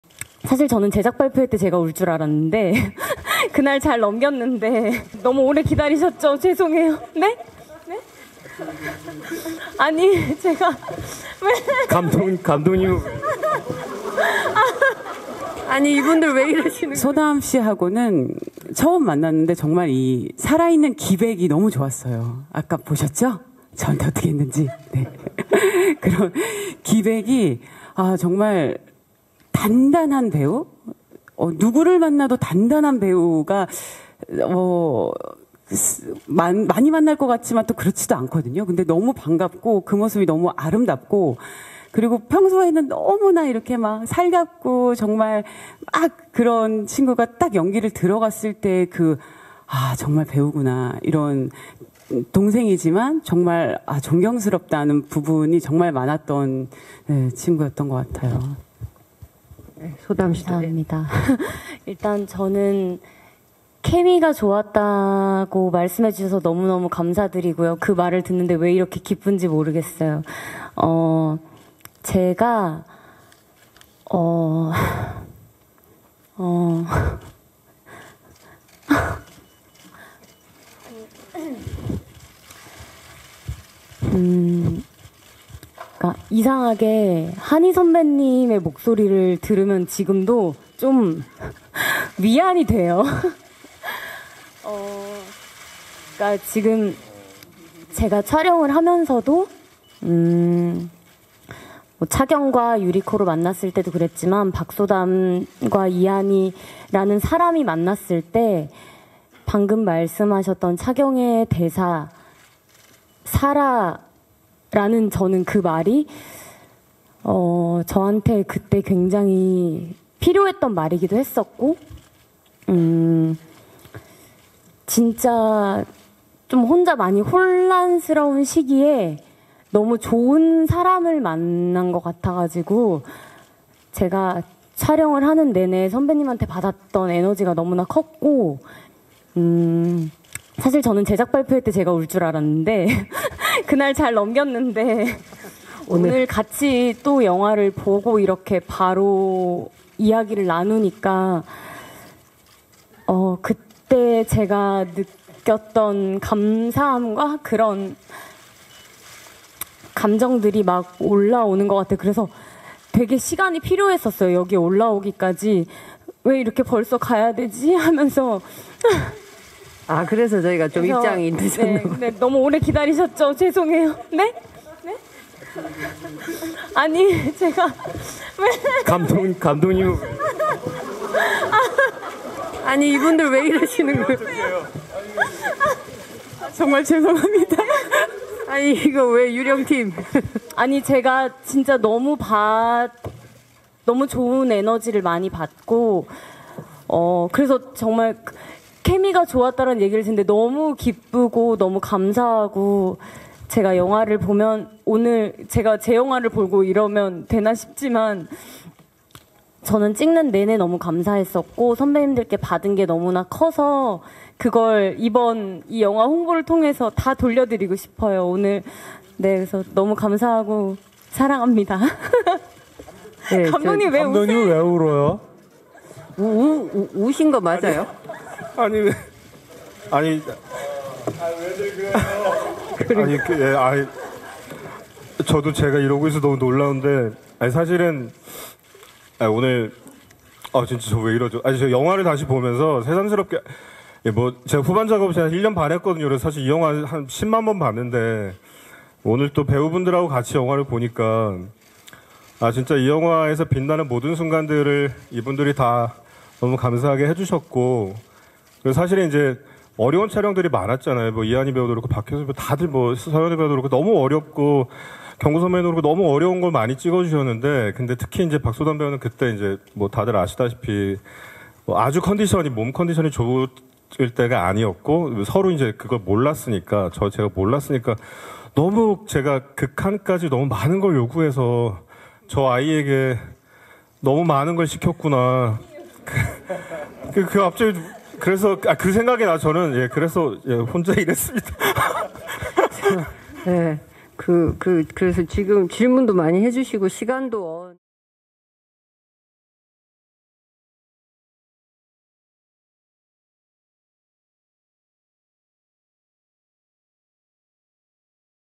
박소담 투병 후 첫 시사회에서 울컥... 갑자기 감독도 같이 운 이유는??
배우 서현우, 이하늬, 박소담, 박해수, 설경구가 11일 오후 서울 용산구 CGV 용산아이파크몰에서 열린 영화 '유령' 언론시사회에 참석했다.